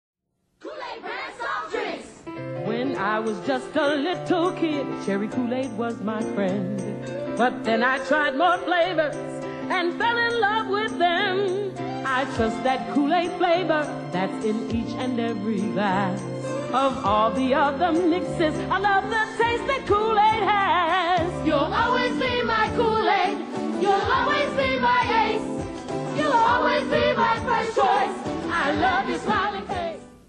Kool-Aid Ad
Kool-Aid Commercial (1992). I remember seeing this commercial every Saturday during Soul Train lol.